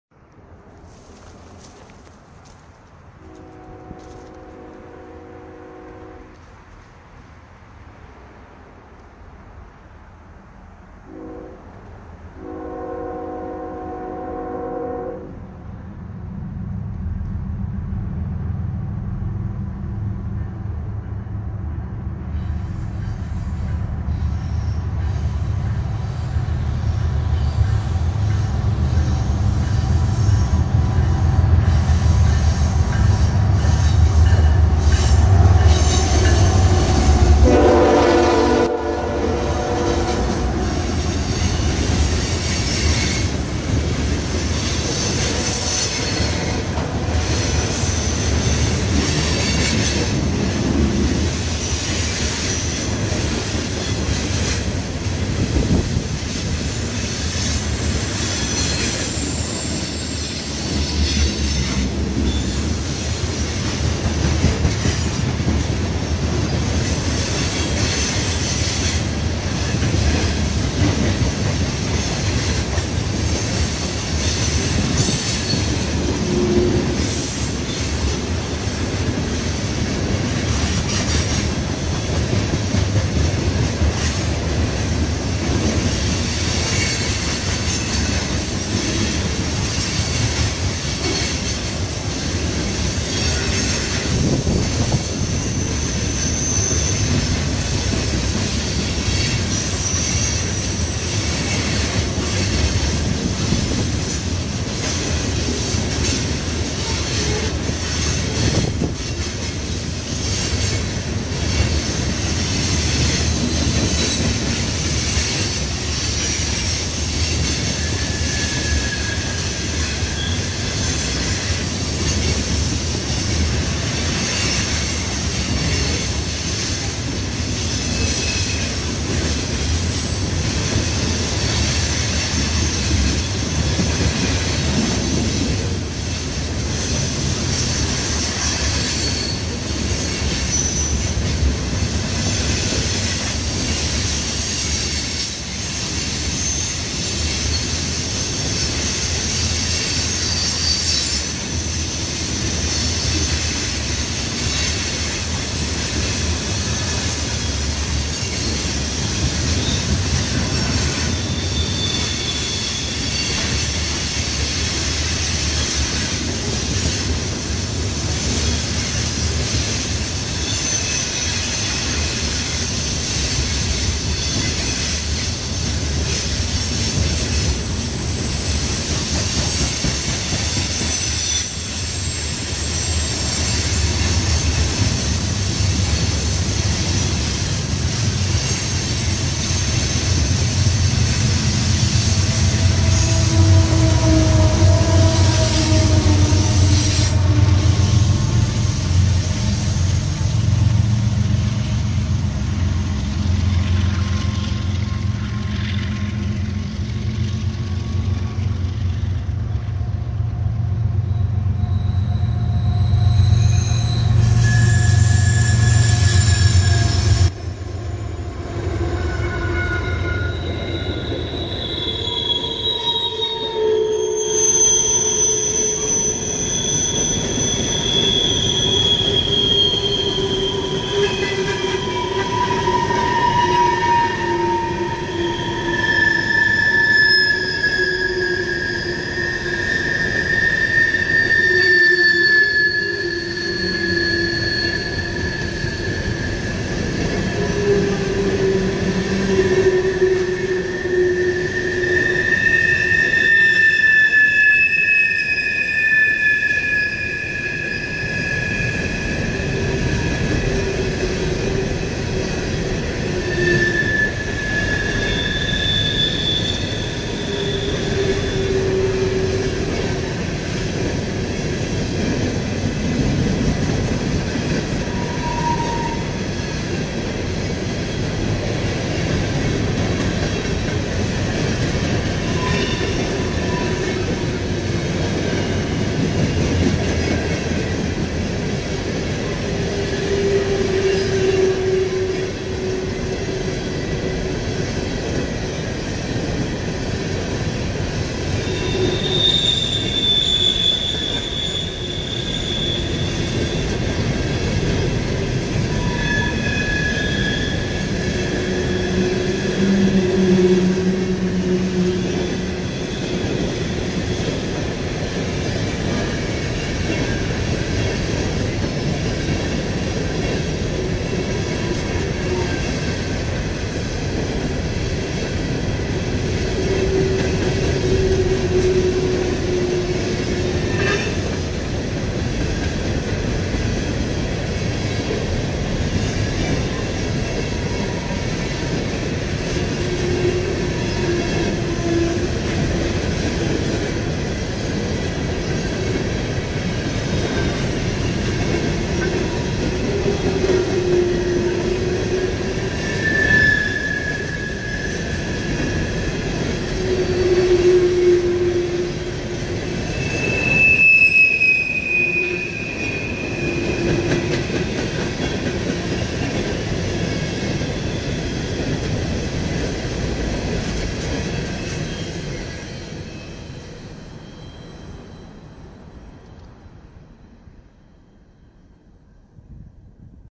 CSX coaltrains head East and West
CSX coal trains head East and West through Point of Rocks.